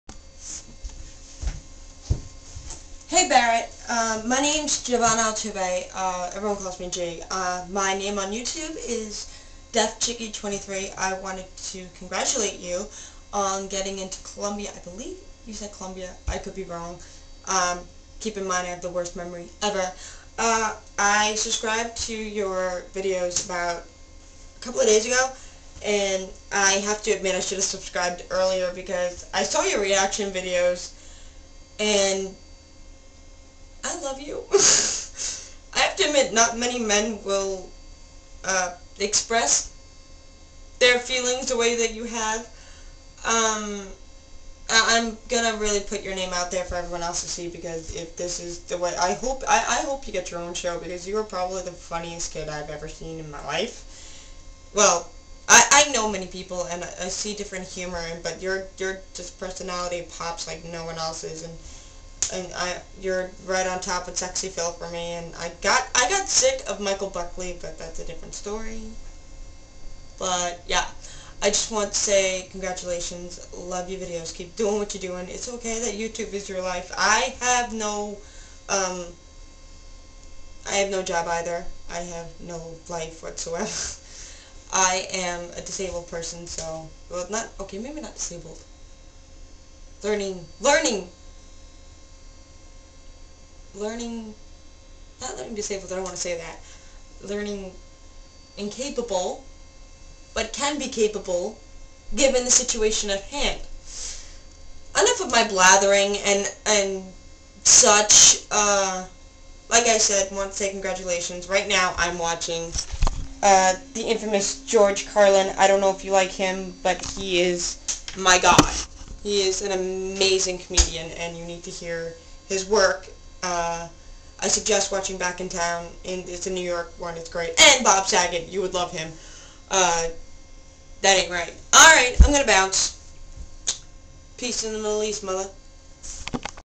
congrats barrett!!!
this is my congrats to barrett for getting into Columbia!